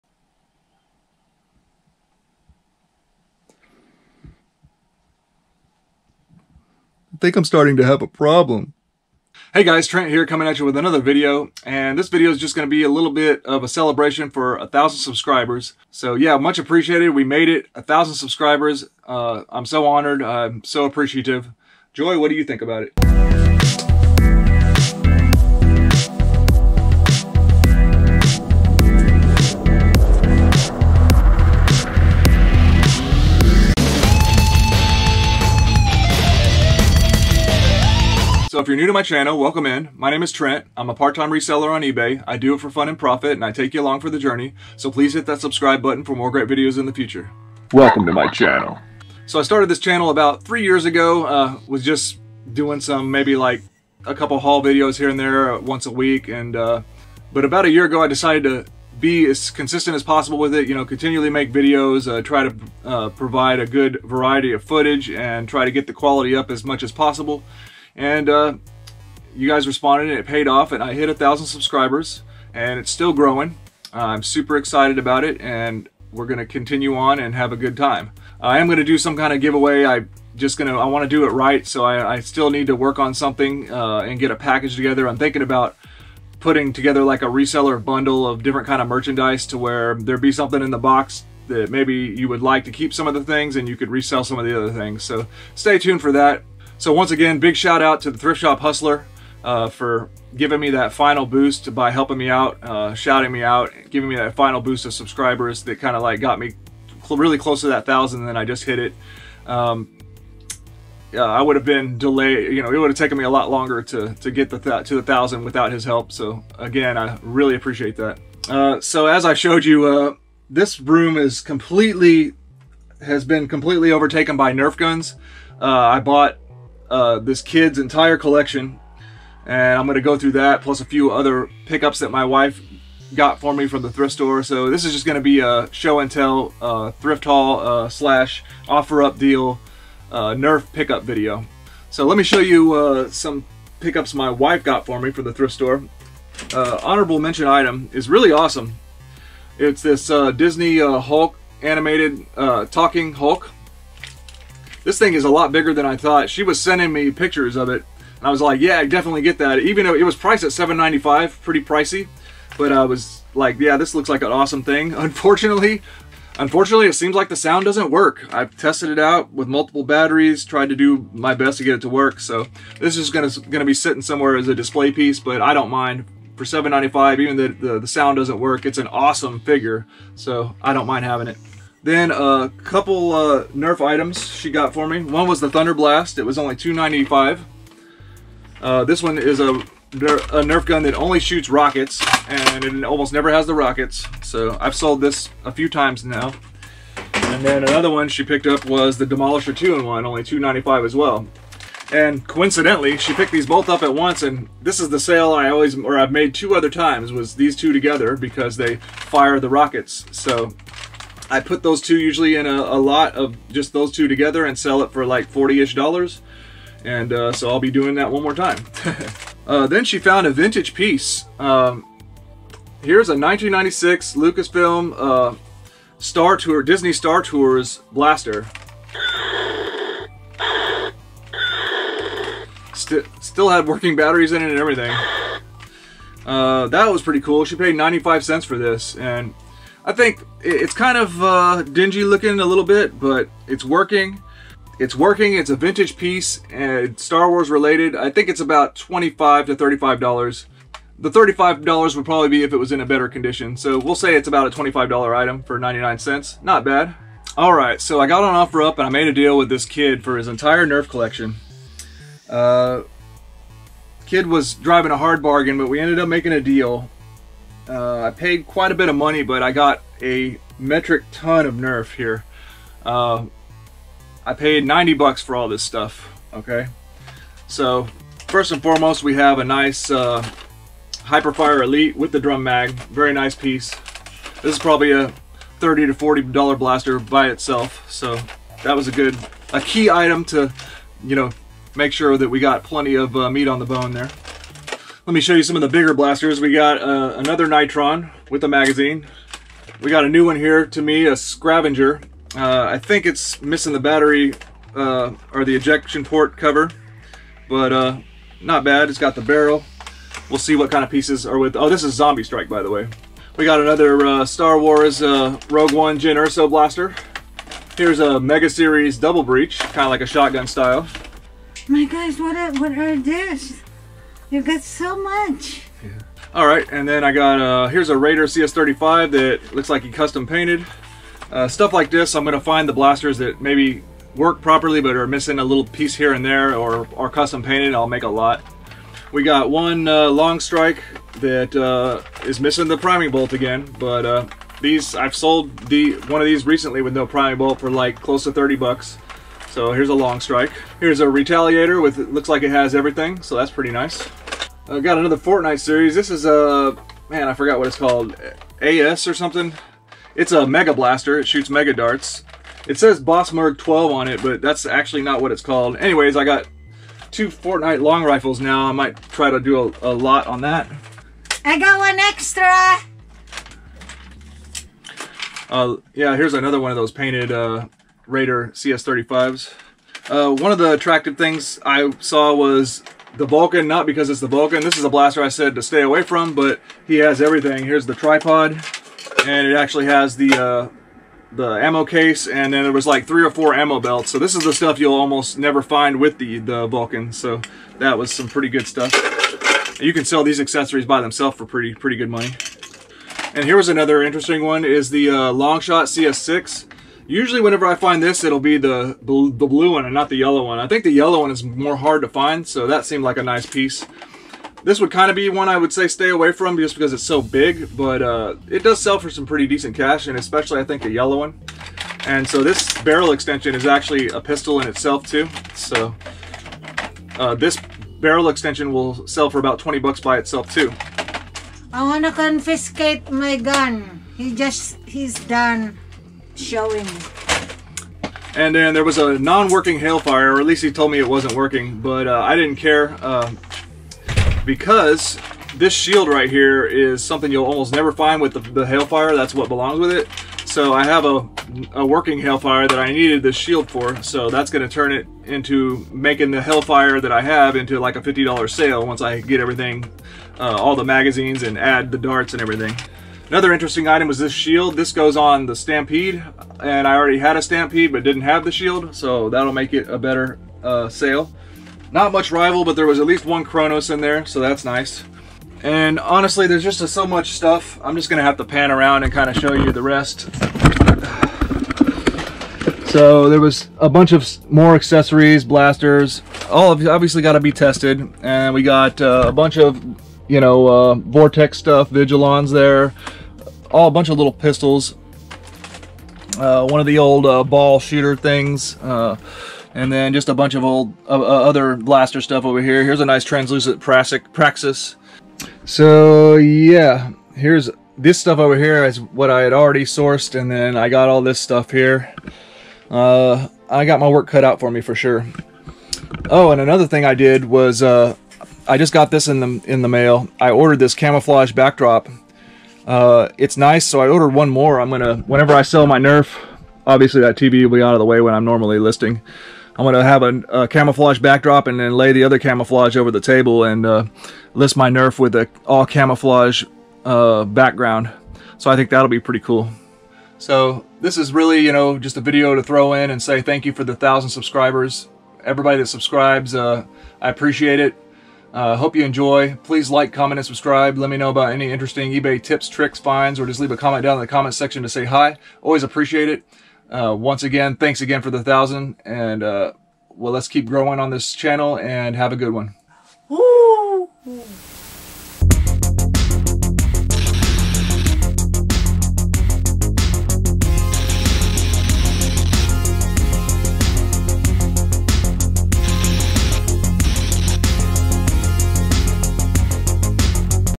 I Bought An Entire NERF Collection To Sell On eBay! Plus Thank You For 1000 Subs!
I Bought An Entire NERF Collection To Sell On eBay!
I got on offerup and ended up buying a huge NERF lot to sell on eBay.

My eBay Store

If you are a Youtube Content Creator and have not installed TubeBuddy yet please use my link to help me out: